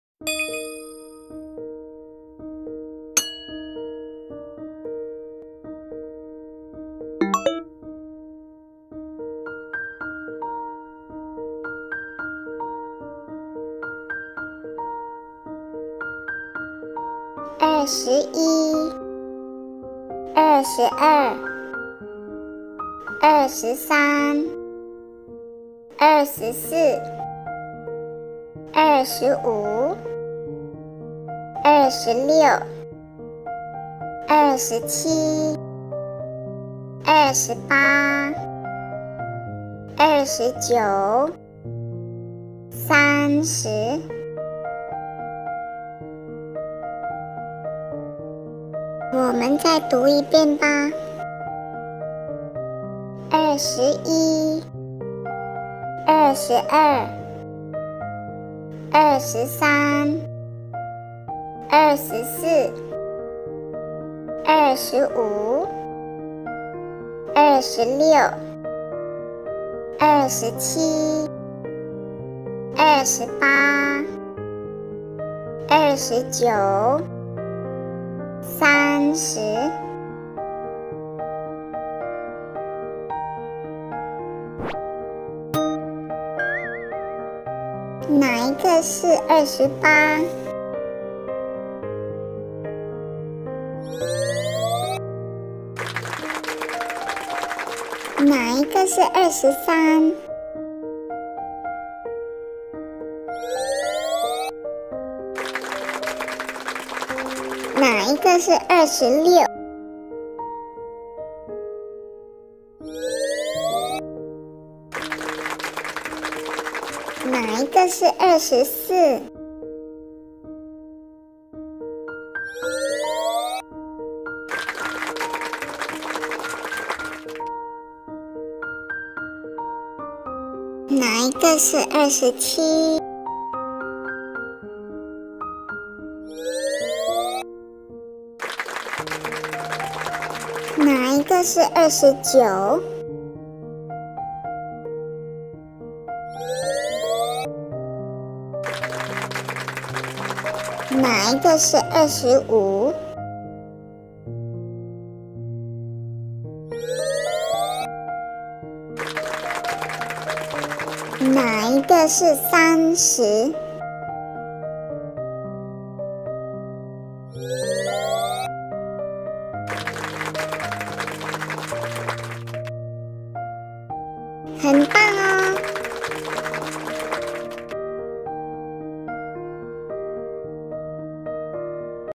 Learn no 21-30 in Chinese | Numbers in Chinese | 数目 | 二十一到三十 | Shu mu
Hello 你好 ni hao.

Let's learn no 21 - 30 in Chinese/Mandarin.